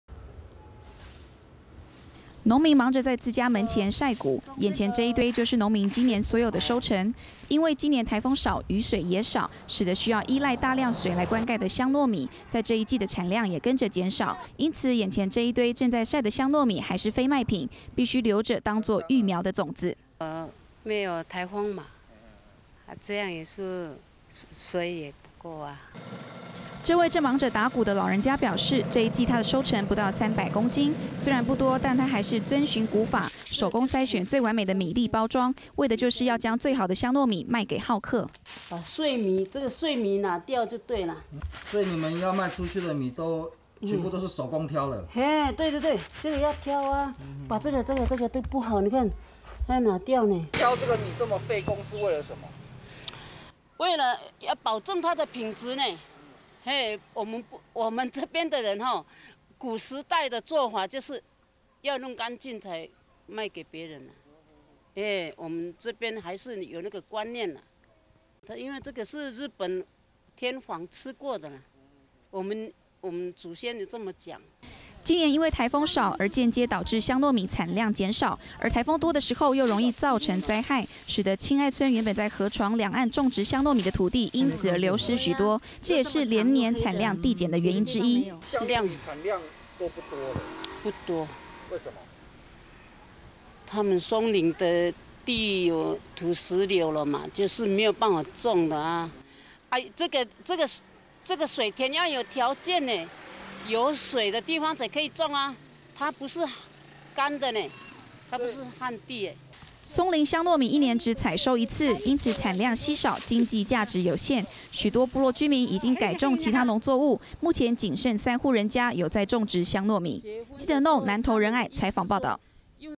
香糯米產量減 親愛村僅存三戶打拼 20111208
【No】

農民忙著在自家門前曬穀，眼前這一堆就是農民今年的所有收成，因為今年颱風少雨水也少，使得需要依賴大量水來灌溉的香糯米，在這一季的產量也跟著減少，因此眼前這一堆正再曬的香糯米還是非賣品，必須留著當做育苗的種子。

這位正忙著打穀的老人家表示，這一季她的收成不到三百公斤，雖然不多，但她還是遵循古法，手工篩選最完美的米粒包裝，為的就是要將最好的香糯米賣給饕客。

今年因為颱風少而間接導致香糯米產量減少，而颱風多的時候又容易造成的災害，使得親愛村原本在河床兩岸種植香糯米的土地因此而流失許多，這也是連年產量遞減的原因之一。

松林香糯米一年只採收一次，因此產量稀少經濟價值有限，許多部落居民已經改種其他作物，目前僅剩三戶人家有在種植香糯米。